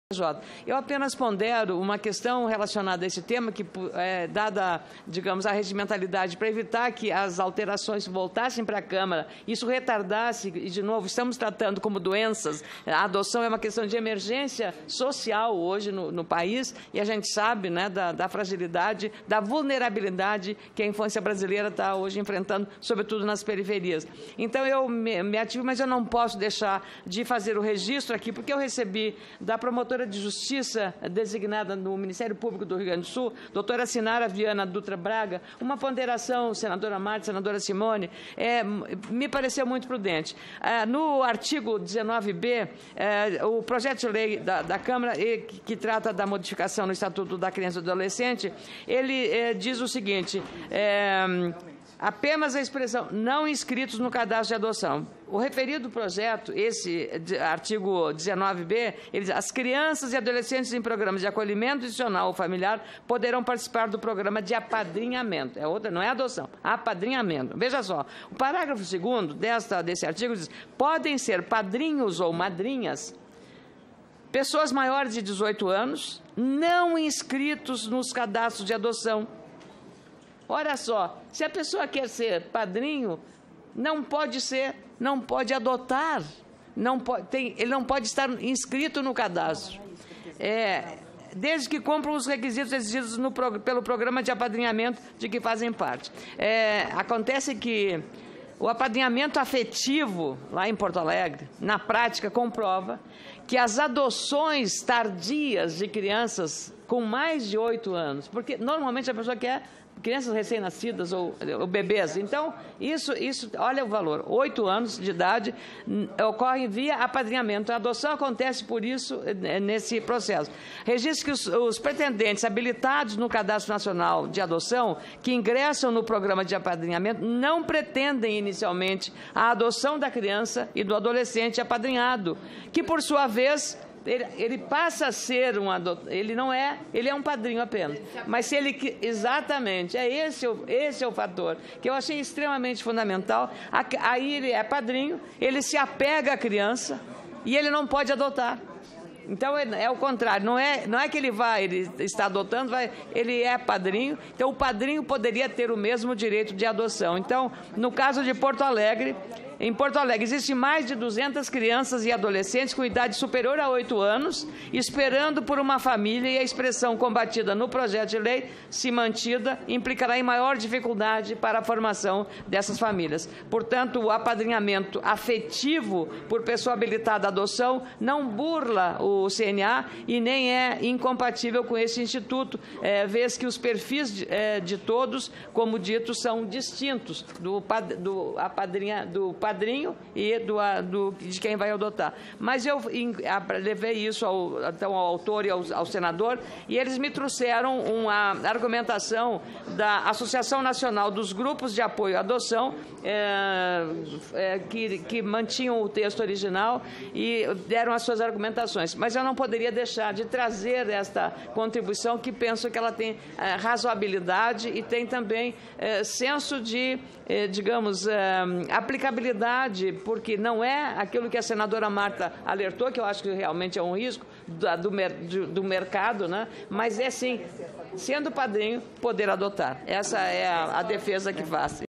Senadora Ana Amélia Lemos destaca o trabalho de promotora sobre a nova Lei de Adoção
Em discurso, na última quinta-feira, 25, no plenário do Senado, em Brasília, a senadora Ana Amélia Lemos destacou o trabalho desenvolvido pela promotora de Justiça da Infância e da Juventude Cinara Vianna Dutra Braga, no que se refere às questões relacionadas aos processos de adoção.